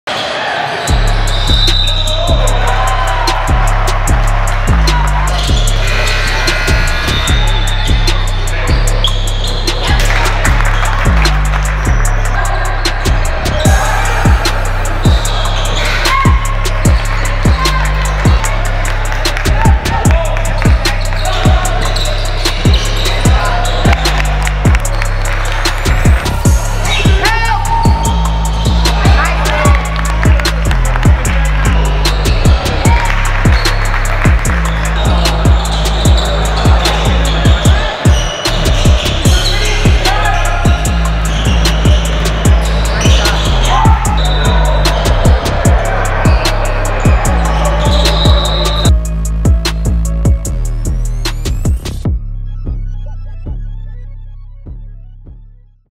Anthony Galvin | Barnstormers 2025
Anthony Galvin highlights from the Prep Hoops Live 2022.